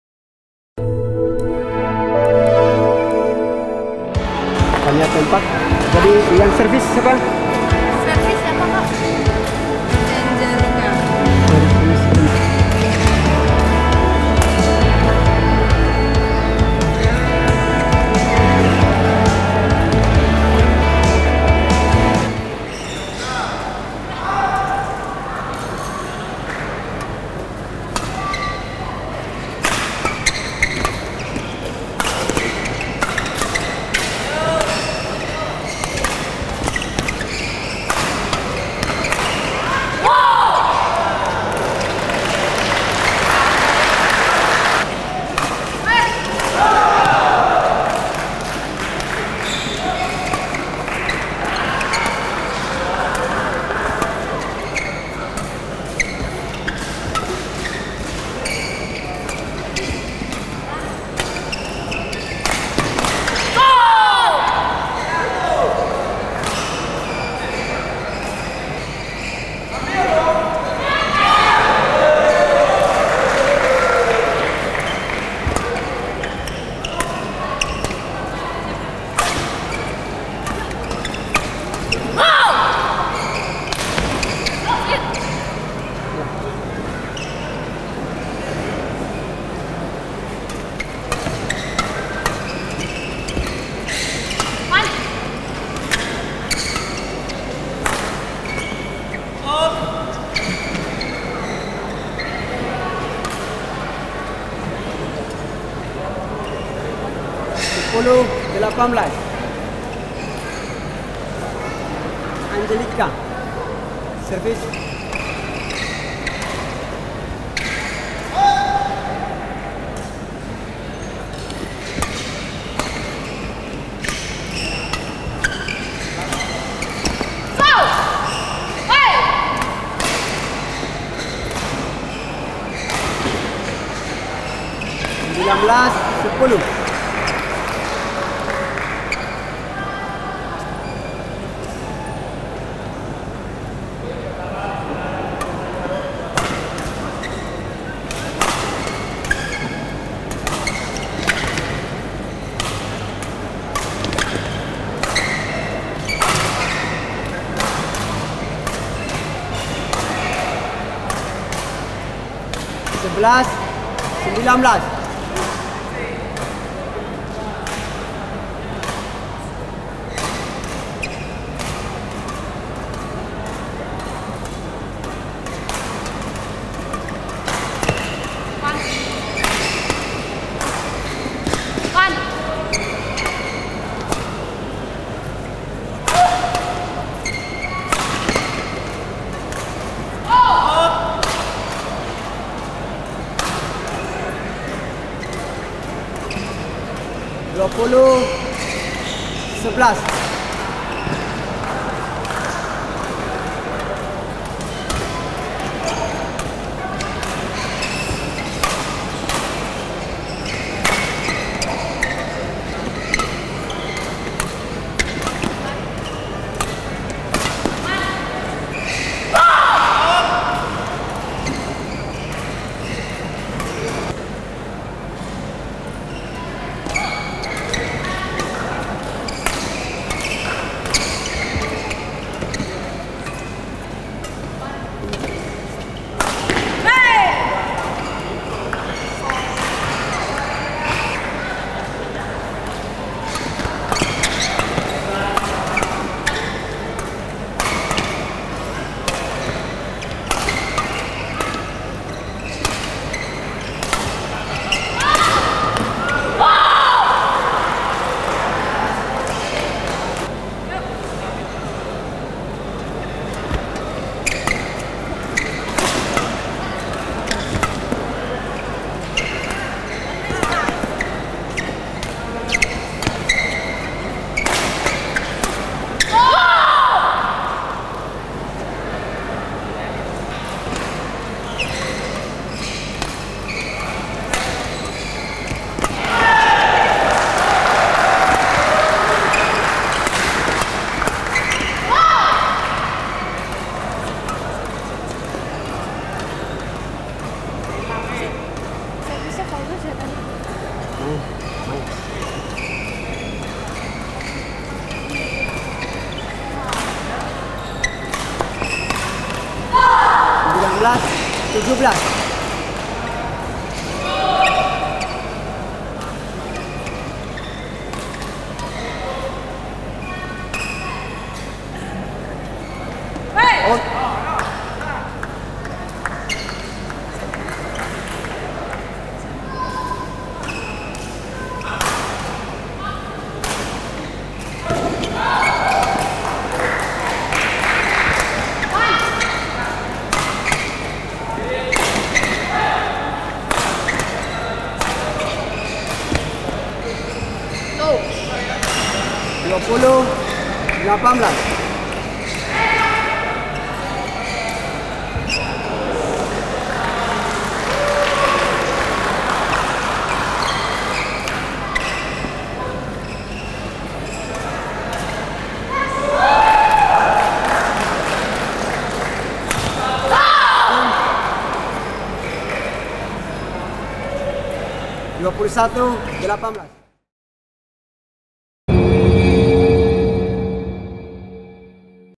Tania Oktaviani K/Vania Arianti S (Djarum Kudus) VS Aghisna Fathul L/Anggelica N (Semen Gresik)
Tania Oktaviani Kusumah/Vania Arianti Sukoco (Djarum Kudus) VS Aghisna Fathul Laili/Anggelica Natasha (Semen Gresik)
Babak Kuarter Final Ganda Taruna Putri Djarum Sirkuit Nasional Nusa Tenggara Barat Open 2015.
26 - 31 Oktober 2015, GOR 17 Desember - Mataram